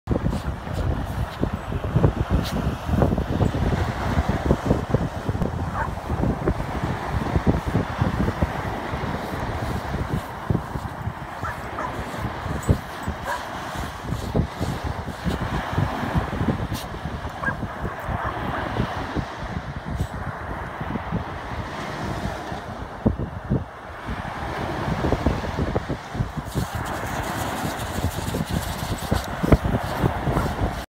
Franklin the Dachshund Digging in the Sand
Here's a video of frank the Dachshund digging in the sand